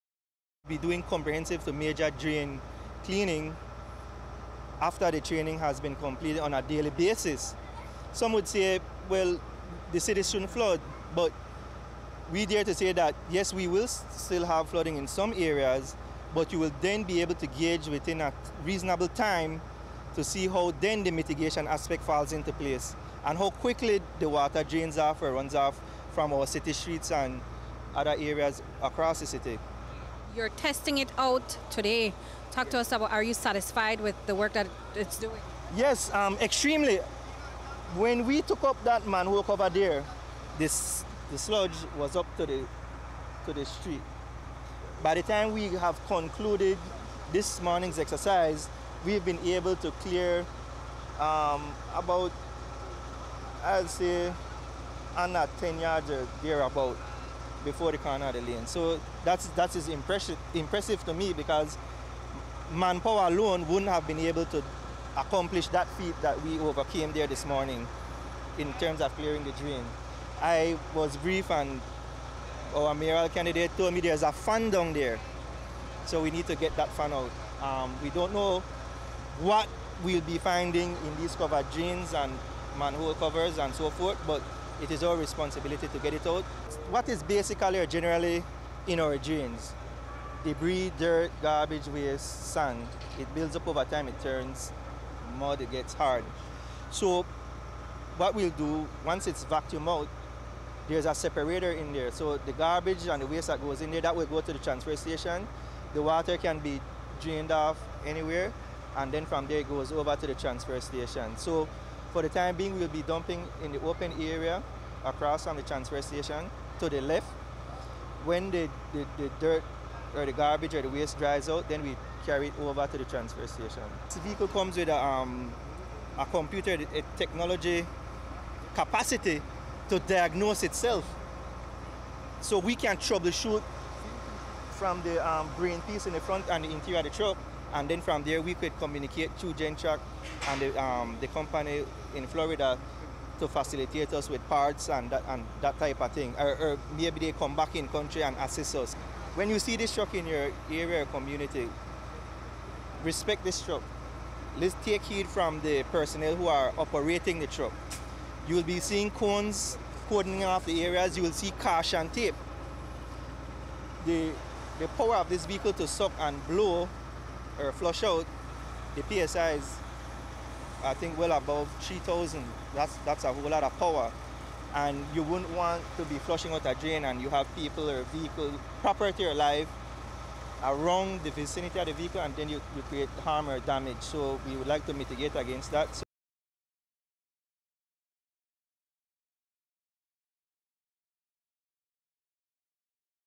City Council turns on jet vacuum drain cleaner
CEMO was out in the City today testing its brand new 2017 jet vacuum cleaner. The high tech piece of equipment will be used to clean out the drains and canals around the city to lessen flooding. It costs almost a million dollars and to make sure that it serves its purpose, the team responsible for its operation is being trained how to use the machine. We went out to King Street today where CEMO and a team were putting the cleaner to a test. CEMO’s Philip Willoughby says that the truck is part of the wider flood mitigation project for Belize City. Last month the jet vacuum cleaner and several other piece of road and canal maintenance equipment were handed over to the Belize City and Ministry of Works through an I.D.B. funded project. Willoughby tells us how this truck will work to clean the sludge out of the canals in the City.